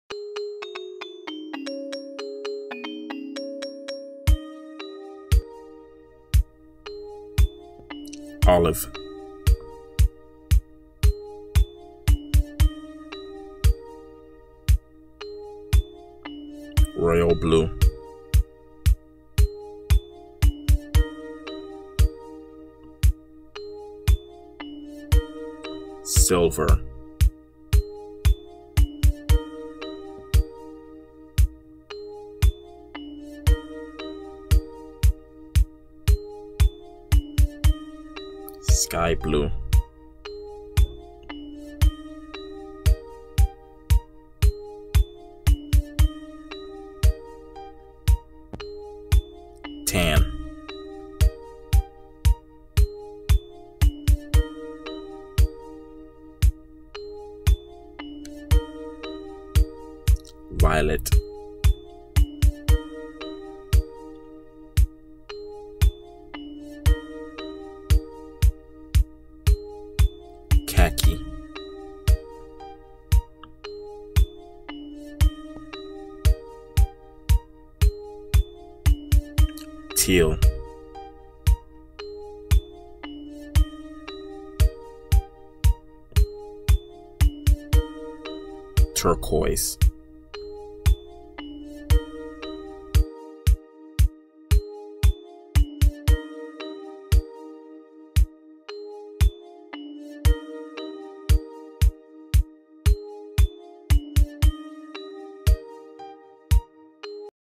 Learning Colors 3
Watch this video to learn more about colors in part 3. A quick and simple video for all ages.

Here are some of our other learning videos.